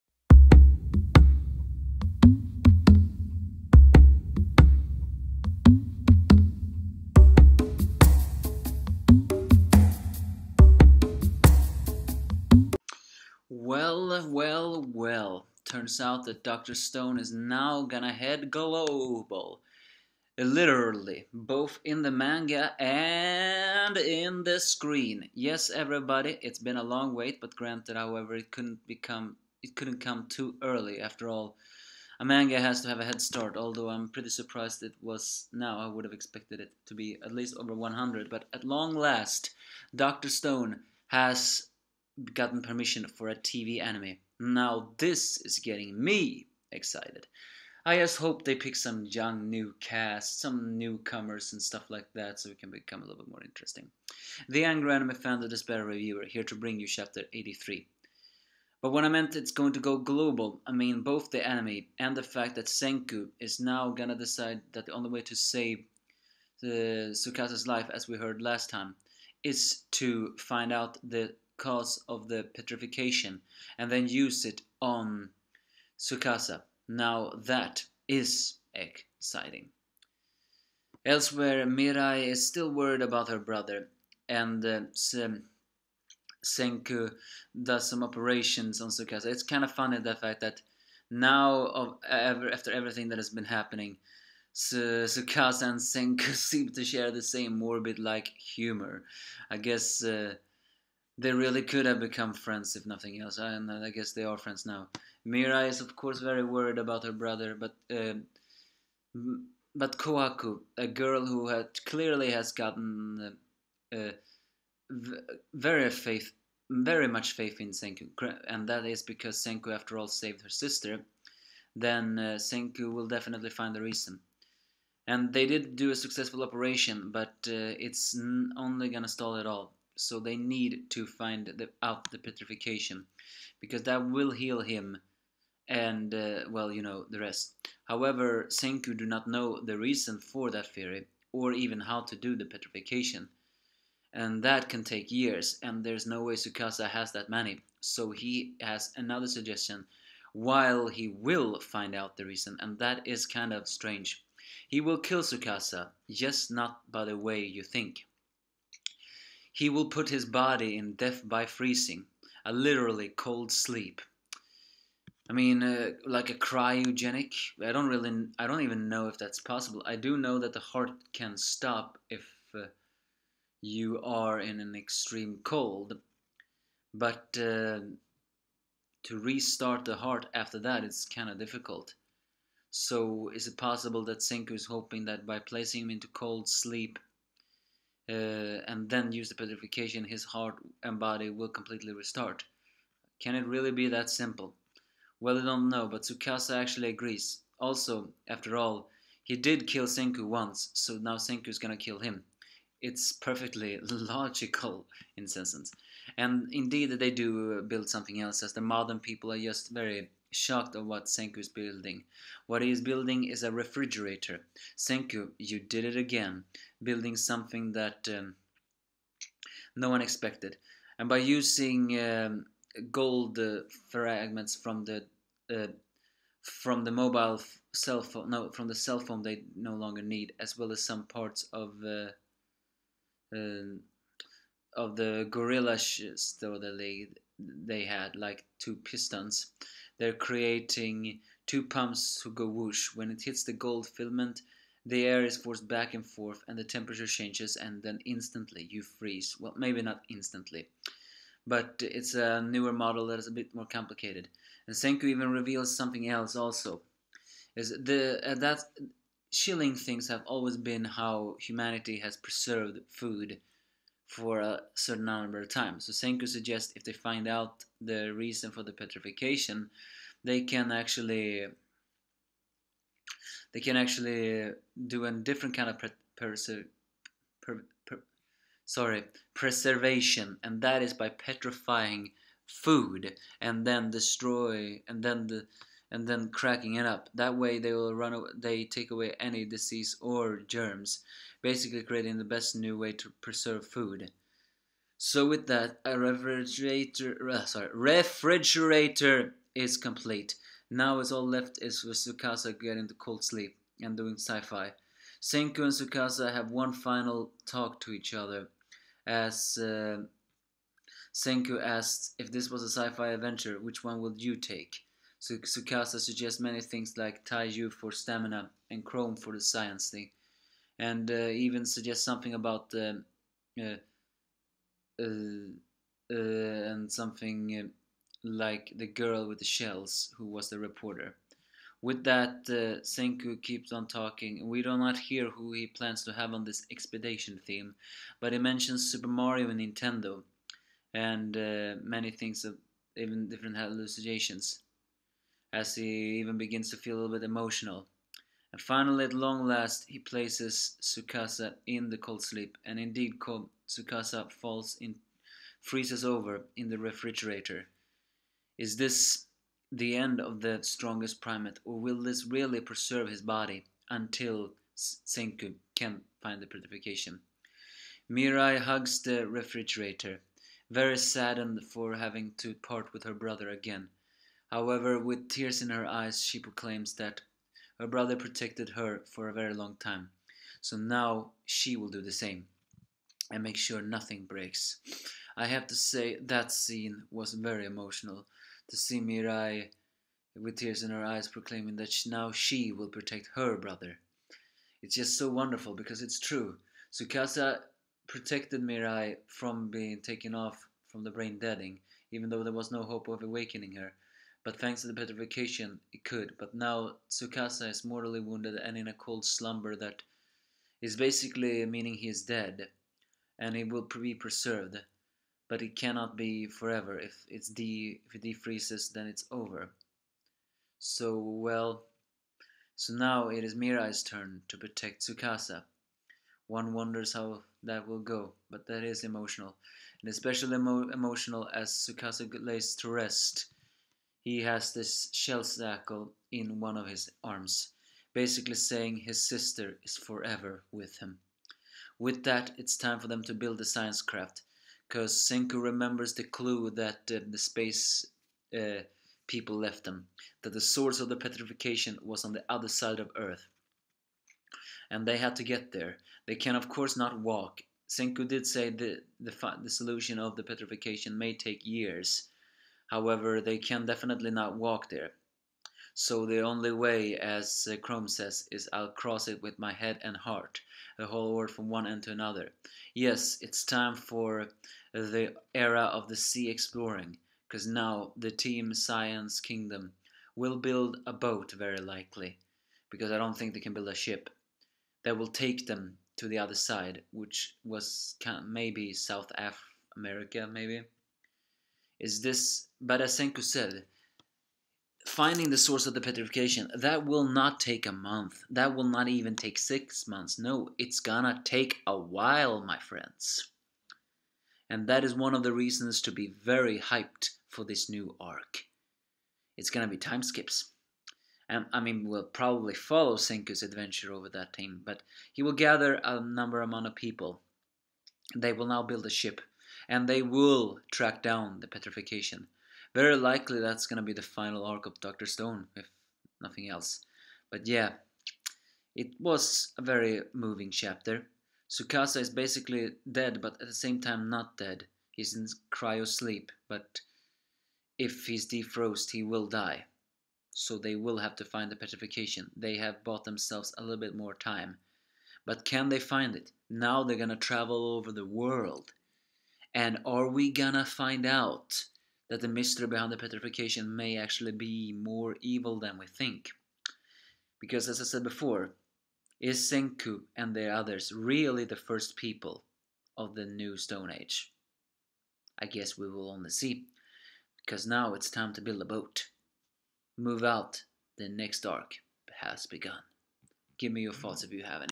Dr Stone Chapter 83 Review Start Of The Third Arc
placing thier new ally in a slumber
the arc starts wiht intetion of going global
manga chapter rating 9/10